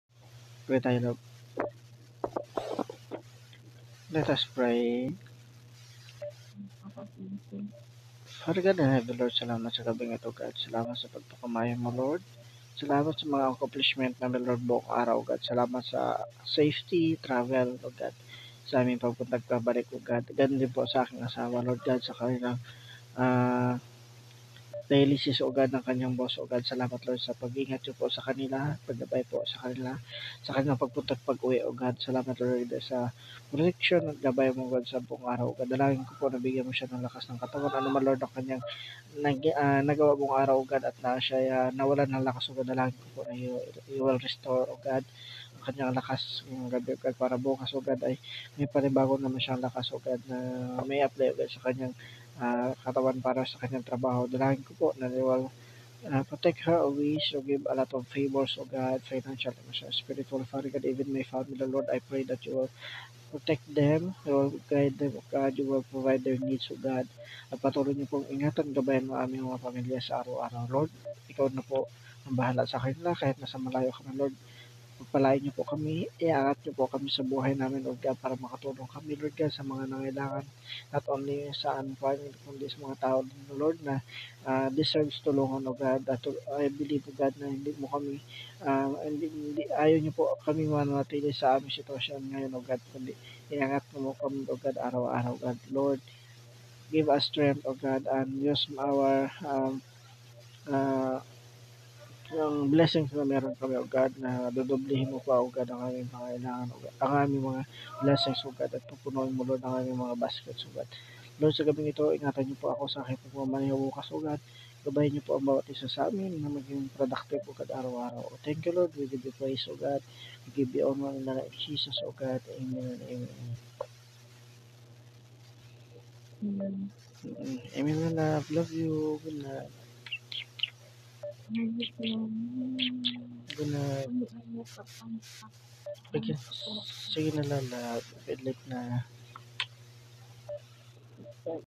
Couple Prayer Every evening Before we Sleep 😴😴😴
Hello guys We pray for everyone and my family and my Wife